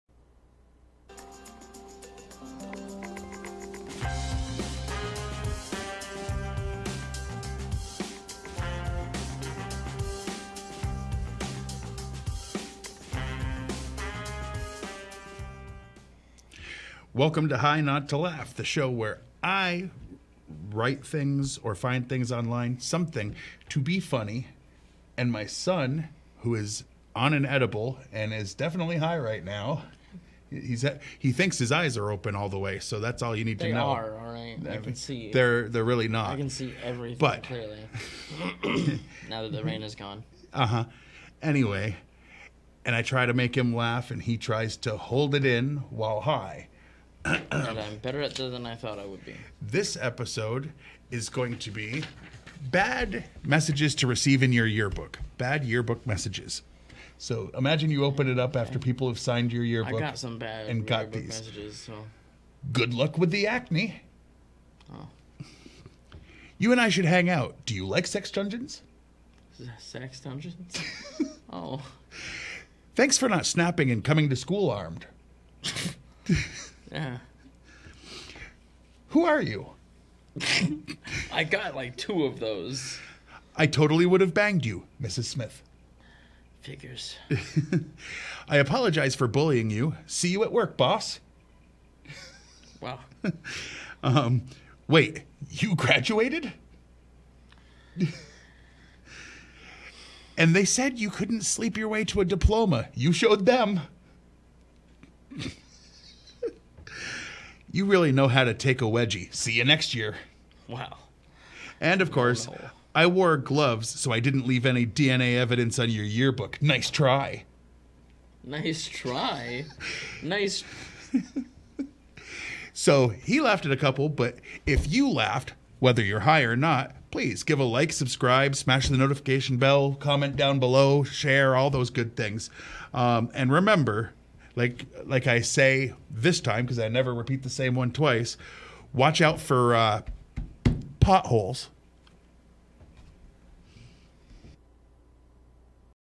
High Not to Laugh: Bad Yearbook Messages
Craig tries to make Jason laugh at funny bad Yearbook Messages.
High not to laugh is a try not to laugh show where a son is on an edible and his dad tries to make him laugh